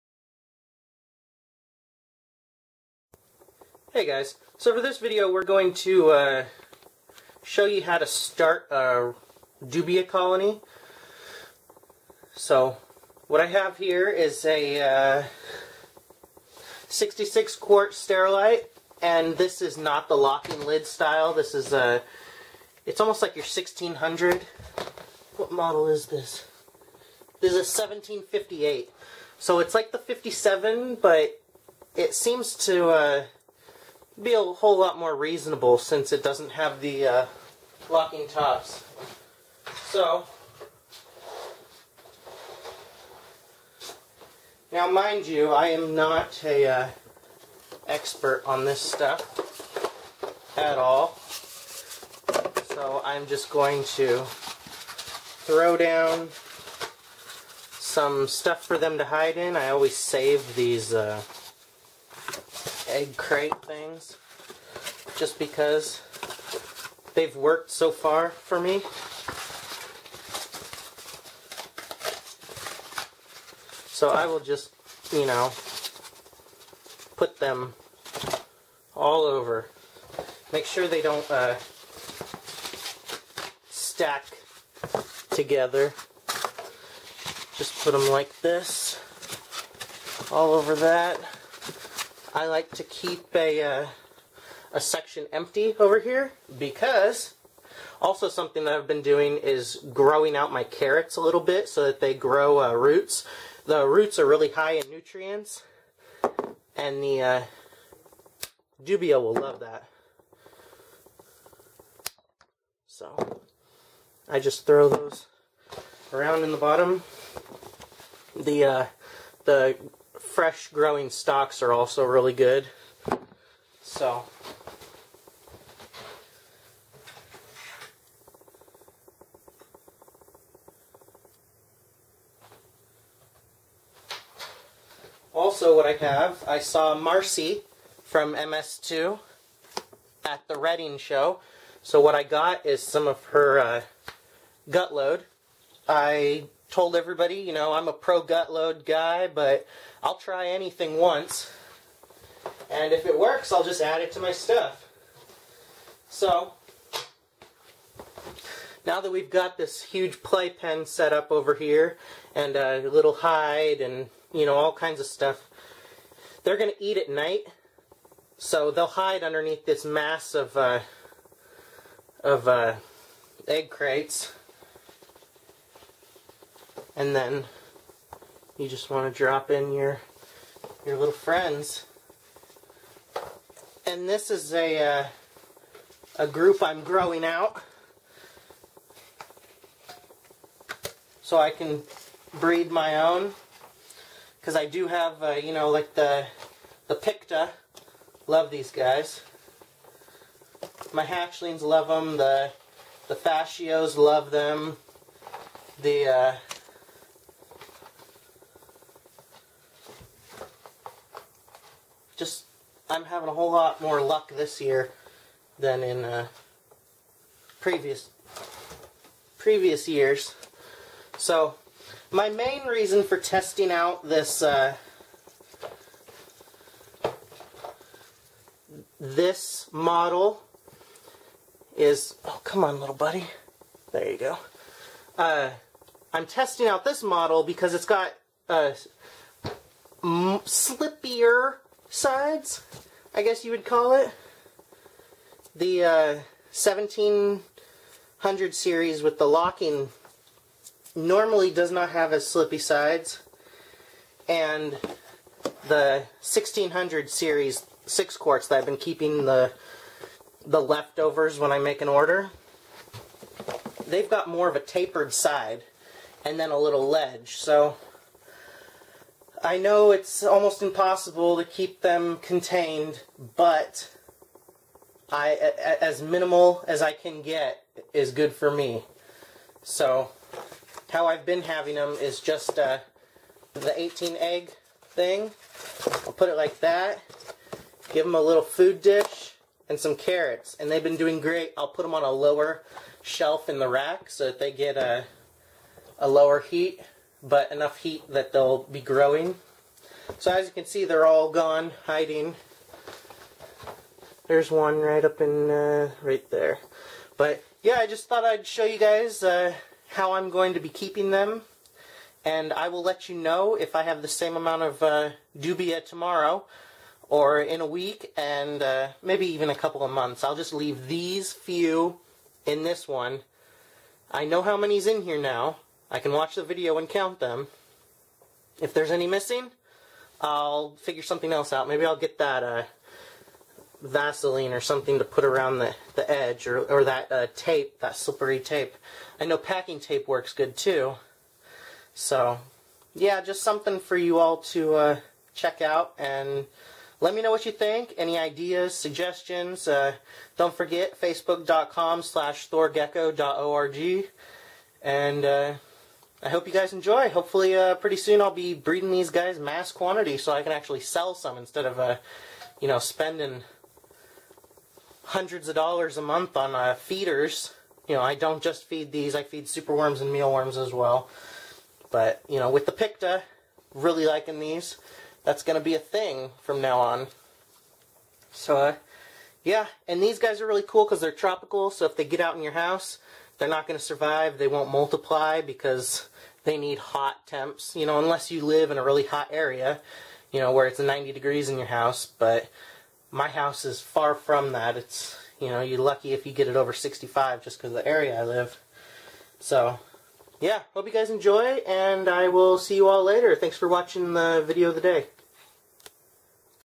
Thor Gecko Video of the Day #31 - SETTING UP DUBIA
How I plan to have my dubia colony.